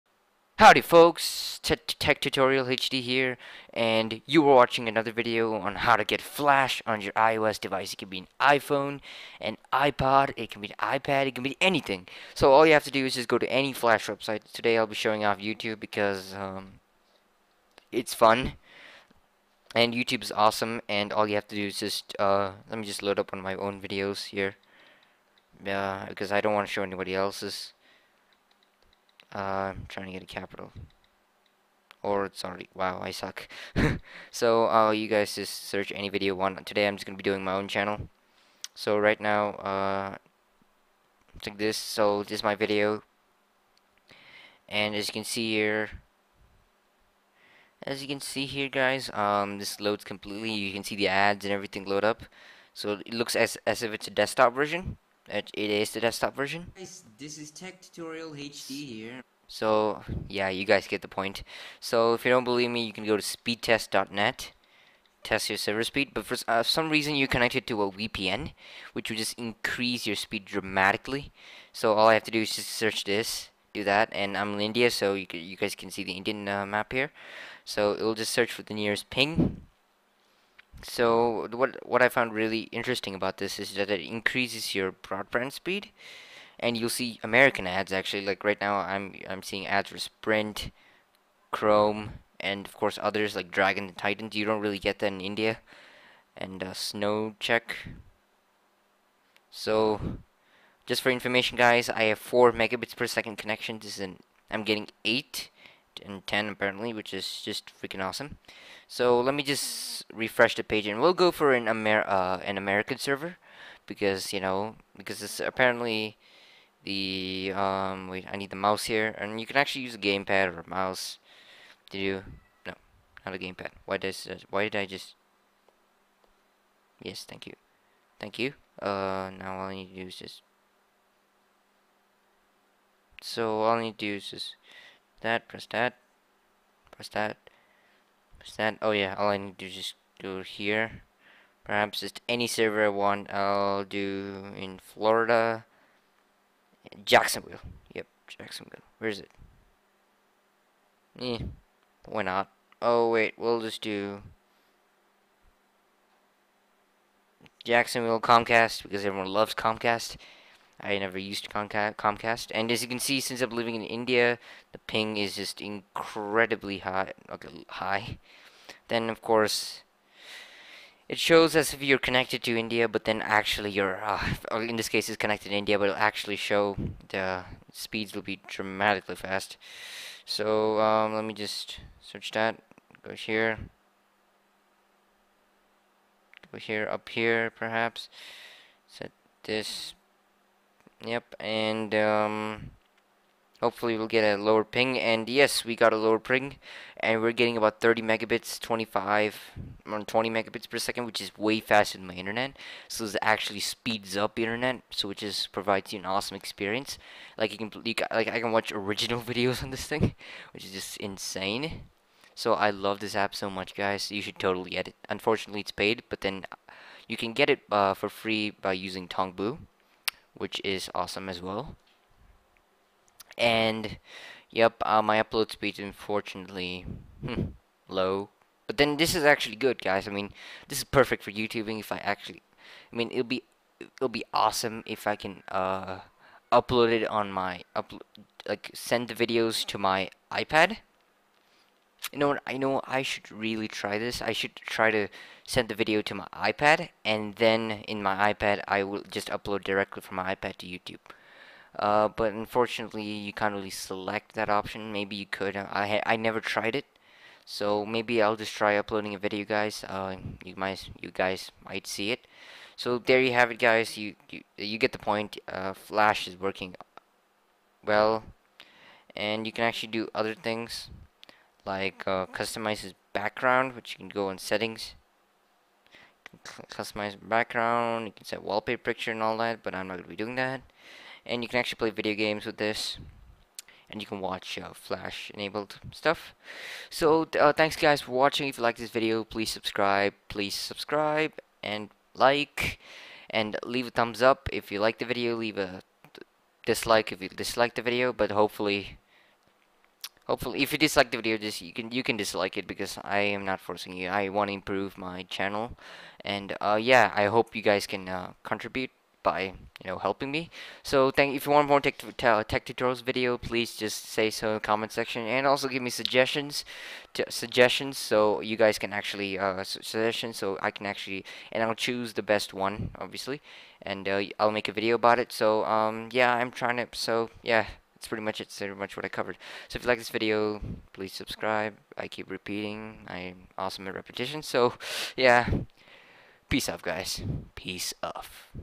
How to Get Flash On iPad/iPhone and iPod iOS 6 & iOS 7- Puffin Web Browser (NO JAILBREAK)
How to get Flash On Your iPhone/iPad and iPod
How to Get Flash On iPad/iPhone and iPod iOS 6 & iOS 7- Puffin Web Browser
Description:
Howdy folks, TechTutorialsHD here. In this video, I will be showing you guys how to get flash on your iOS devices. Thank you guys so much for the support lately. Smash that like button NOW!!